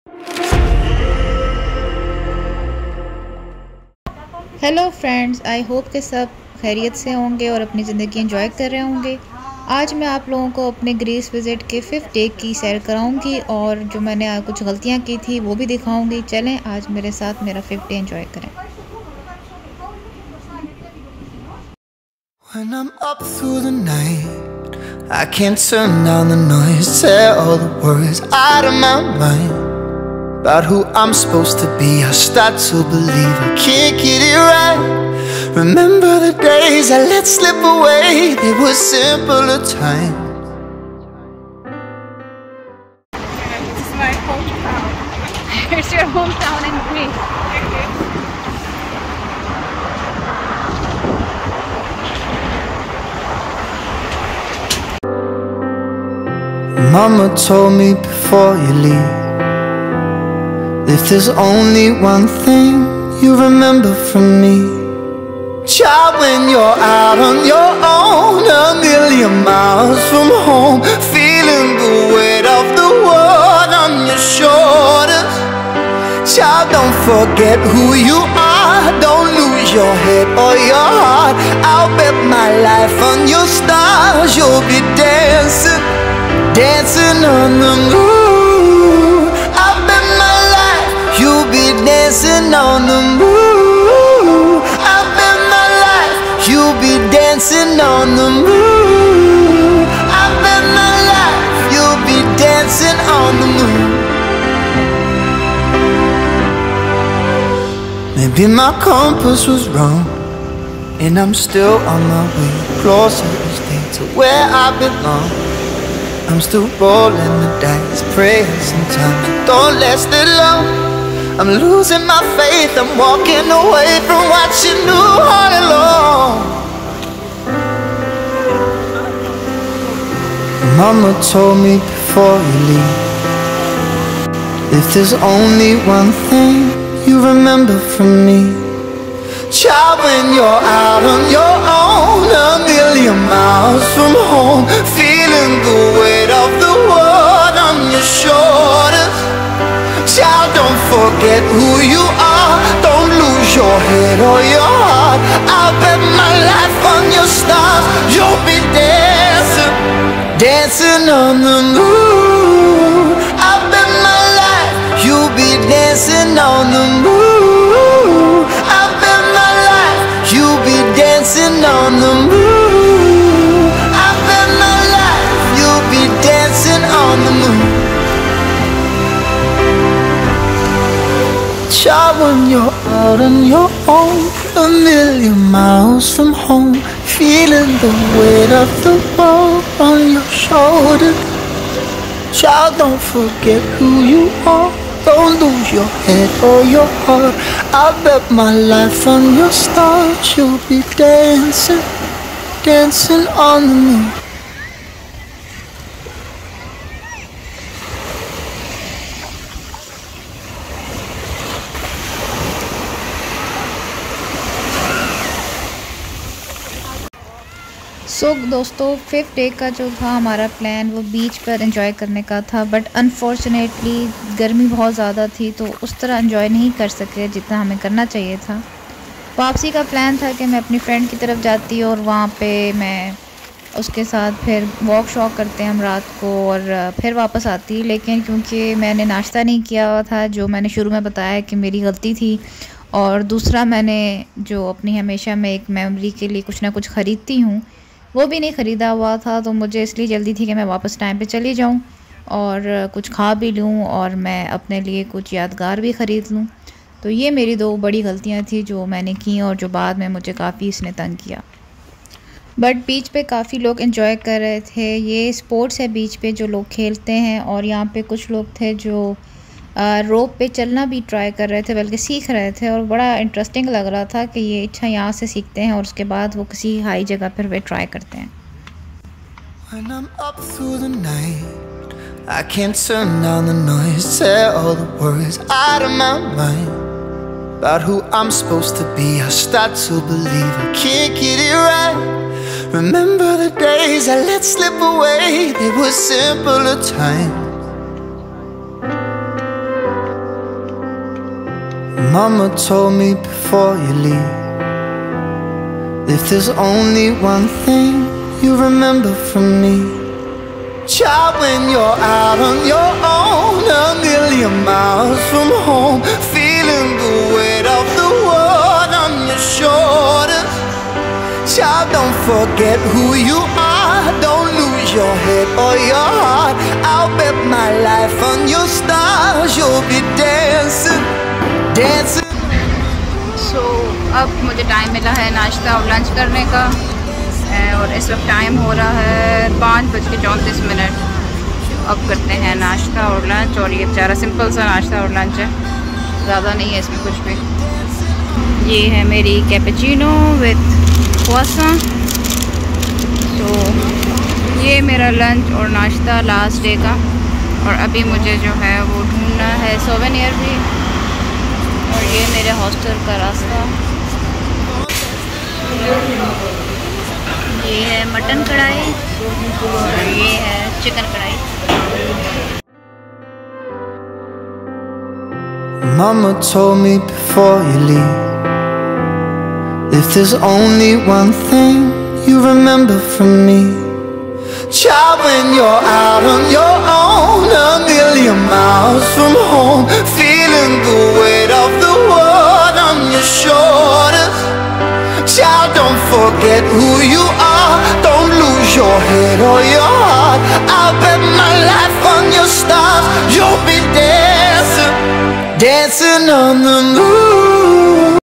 Beach Day in Athens | Day 05 | Greece | What mistakes did I make?
The last day at Athens. We decided to have some fun on beach.